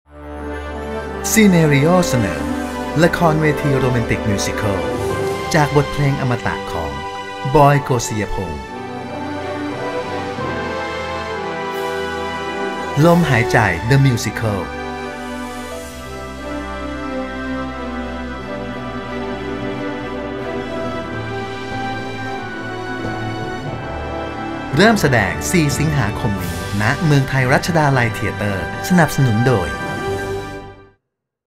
ลมหายใจ เดอะมิวสิคัล
ลมหายใจเดอะมิวสิคัล ละครเวทีที่รวมเพลงรักโรแมนติกมากที่สุด จากเพลงรักของบอยโกสิยพงษ์ สู่ละครเวทีมิวสิคัล นำแสดงโดย กัน-นภัทร โต๋-ศักดิ์สิทธิ์ หนูนา-หนึ่งธิดา และ ปาน-ธนพร
‪‎ลมหายใจเดอะมิวสิคัล‬ เริ่มแสดง 4 สิงหาคม 2559 ณ เมืองไทยรัชดาลัยเธียเตอร์
จองบัตรวันนี้ ที่ไทยทิคเก็ตเมเจอร์